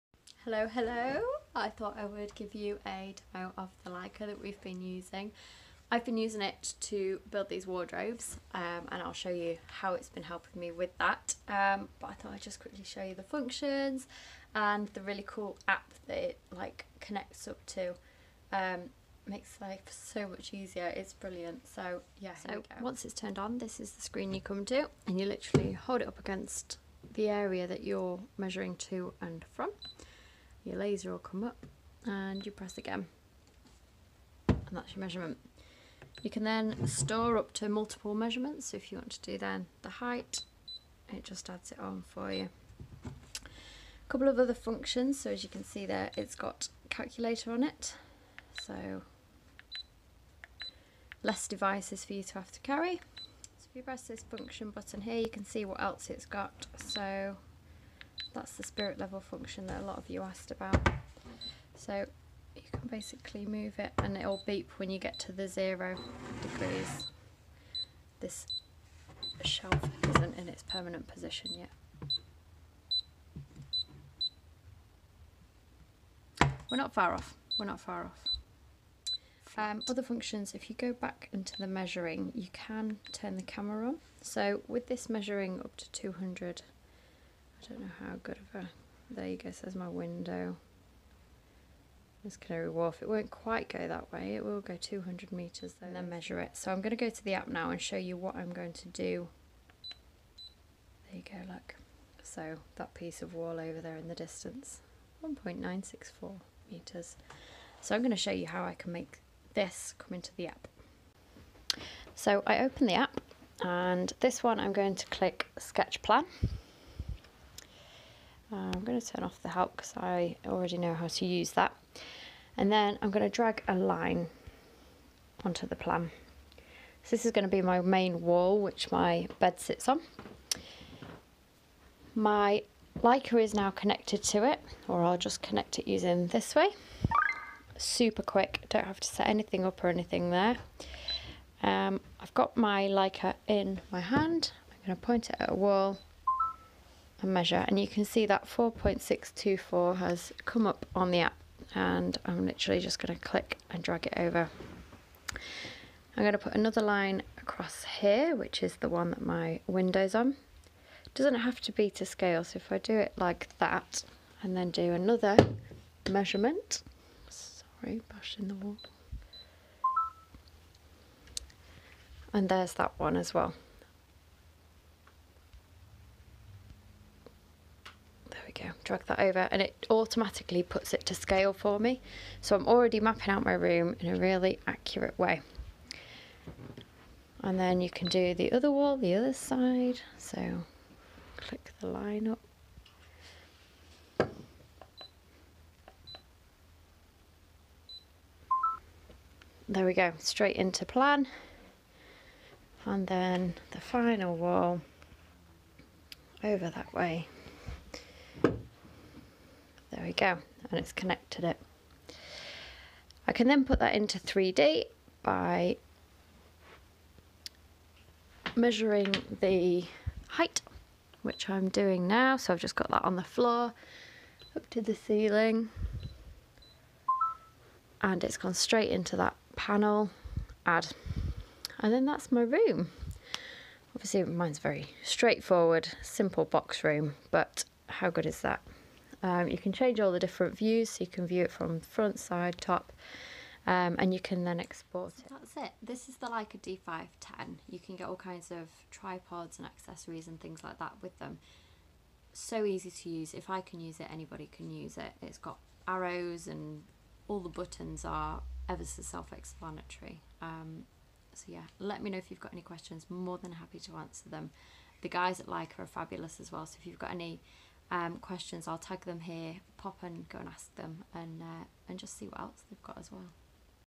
Review of the Leica Disto D510 distance meter
After using the Disto D510 for 3 weeks, I show you the main features that I'm using and how it's saving me time.